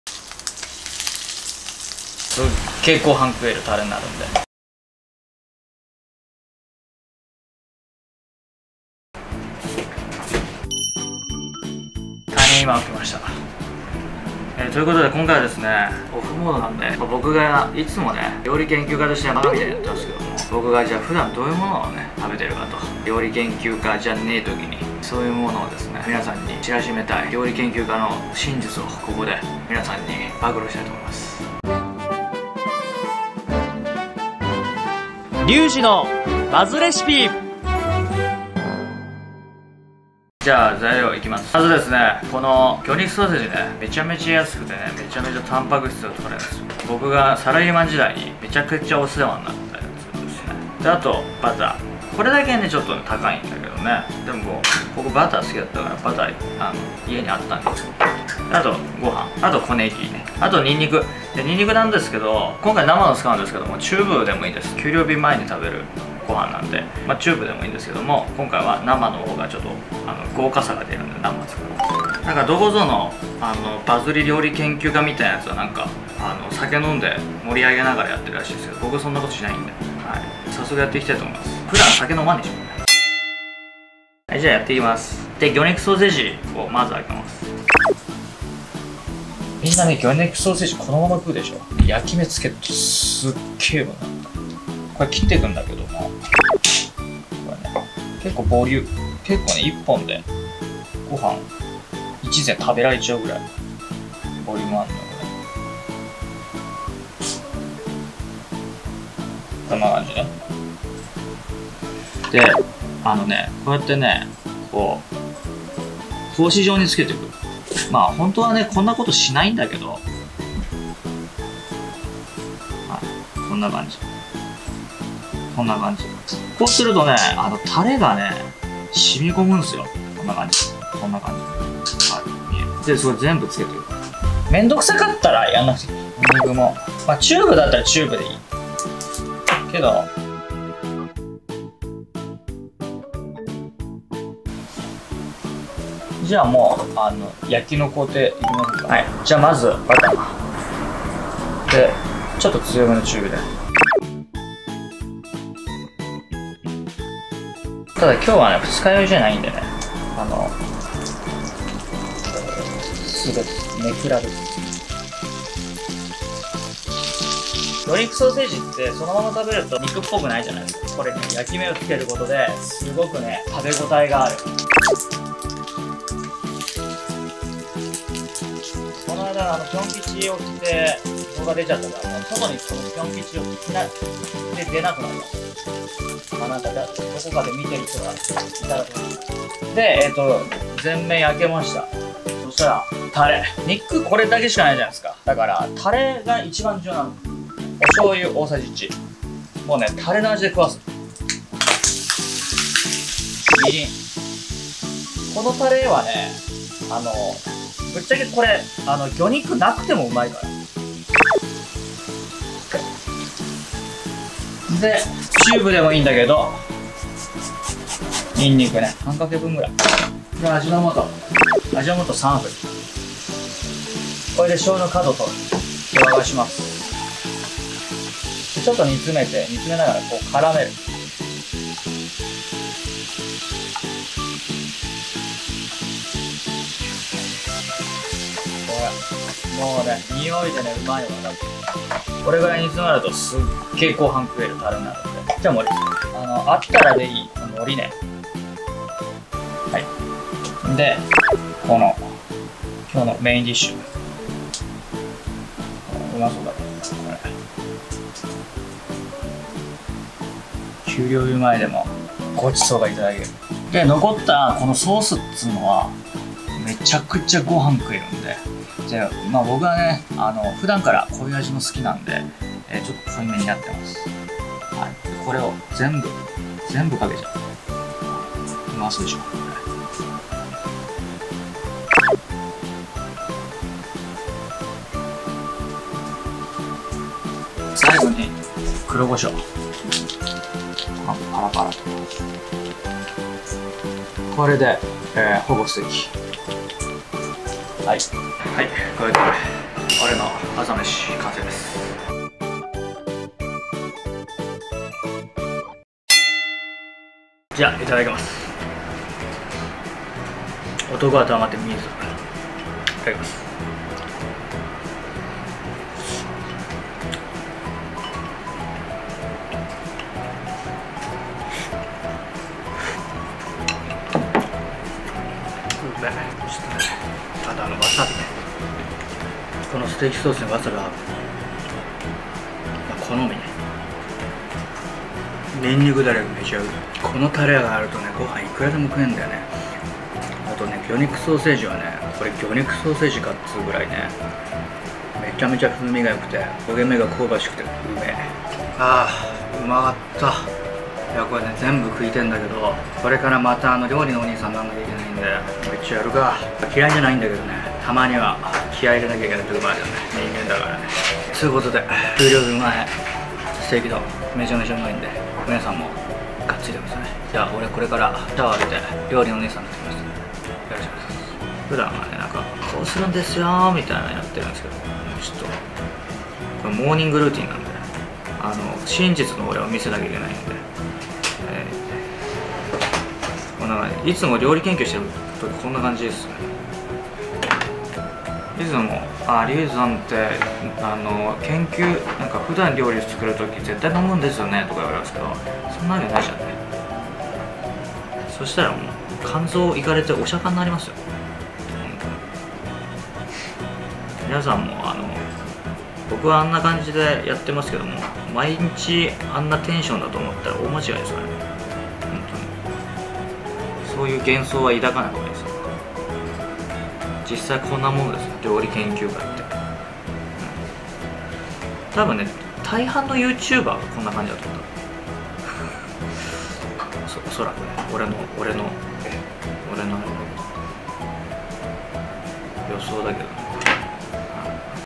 給料日前でも食える最強のコスパ。ほぼ100円で出来る究極のステーキ丼【給料日前ステーキ丼】
【給料日前ステーキ丼】

※動画内でエアコンの風の音が入ってしまって普段より少しうるさく感じるかもです。申し訳ないです。

★今回のレシピはこちら↓
魚肉ソーセージ　1本
ご飯　　　200g
バター　　10g
にんにく　半片
小ねぎ　　適量
黒胡椒　　適量
★味変でわさび

2021年7月21日発売の新刊！

【ワンパンで面倒なし! フライパン飯革命】

【バズレシピ 真夜中の背徳めし】

フリーBGM
DOVA-SYNDROME